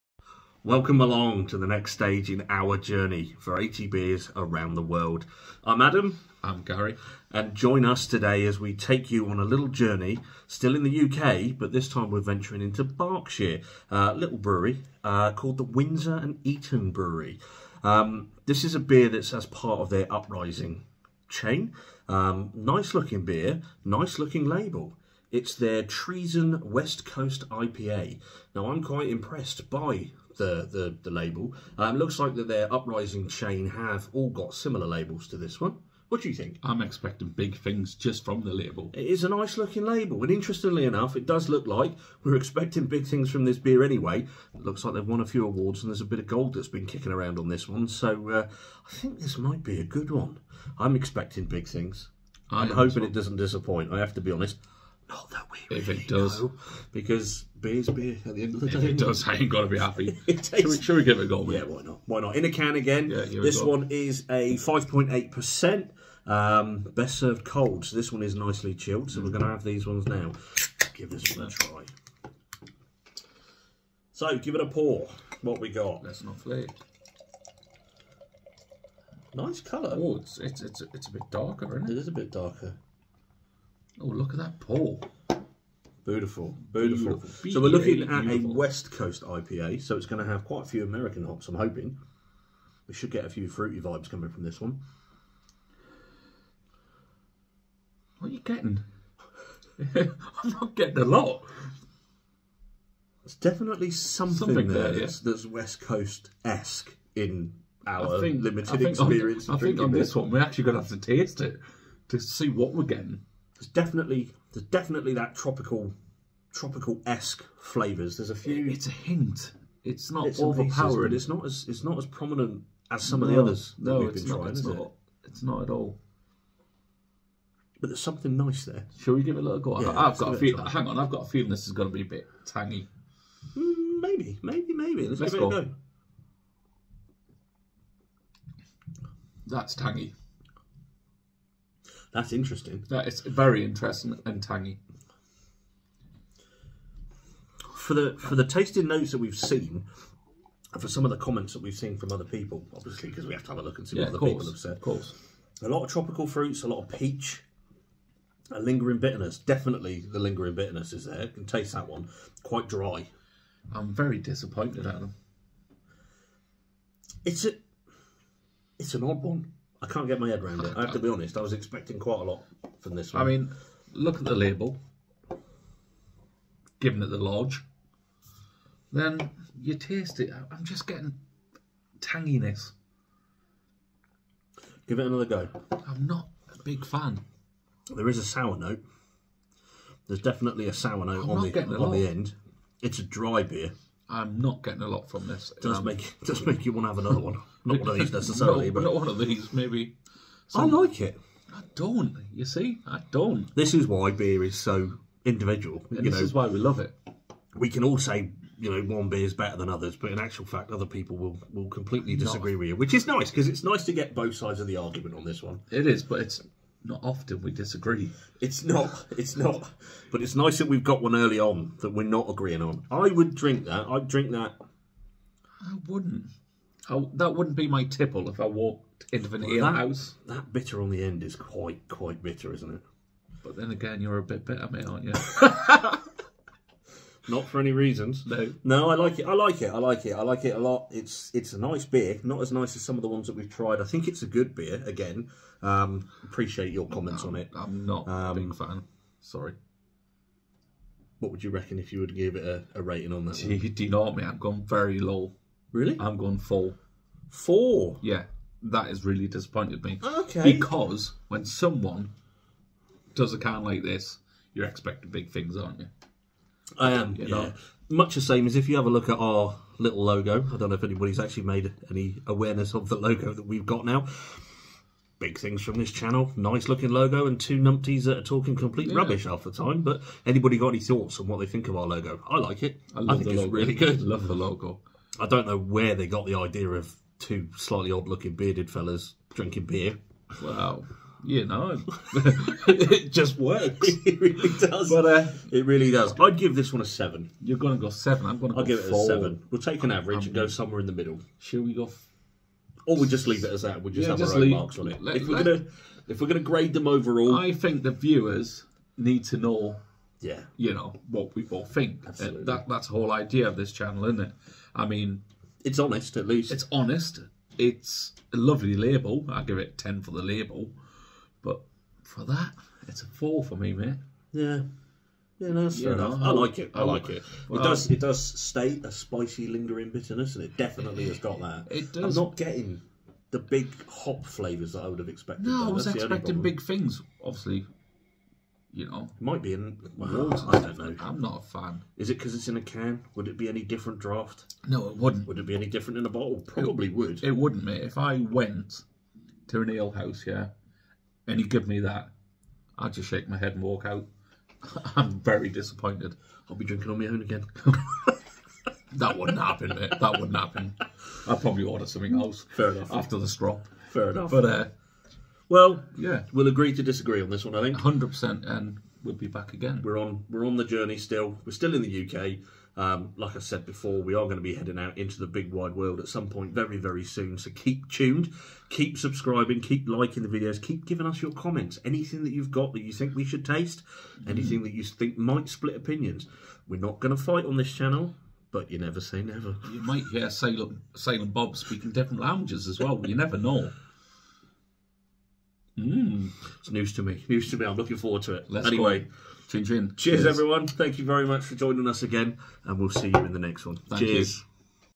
Treason West Coast IPA Review - Around the World in 80 Beers - the next stage.....
Join us as we try/review Treason West Coast IPA from Windsor and Eton Brewery.

This beer certainly divided opinions. take a look and see what we thought.